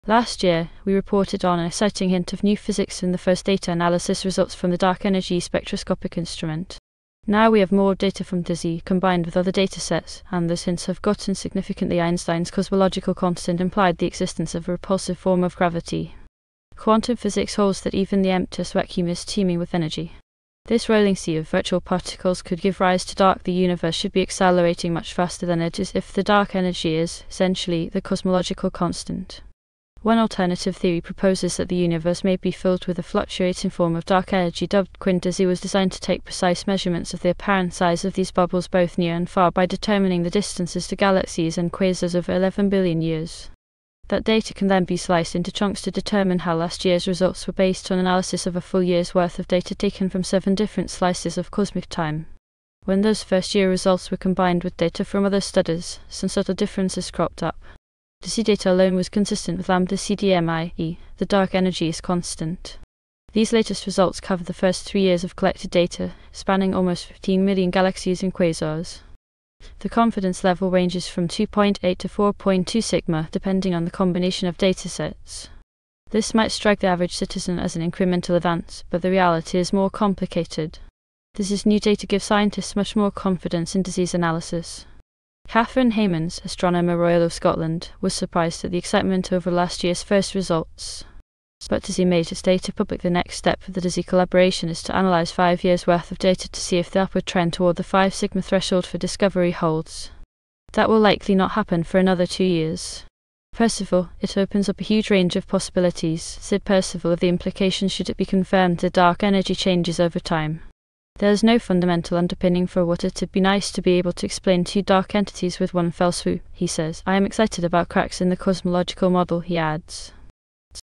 Dark Energy Changes Over Time
Dark Energy or Dark Matter is a type of energy/matter that makes up a massive amount of the universe that we can't see. Analysis of some of this energy throughout the universe has allowed us to learn more about how it interacts with the universe over time.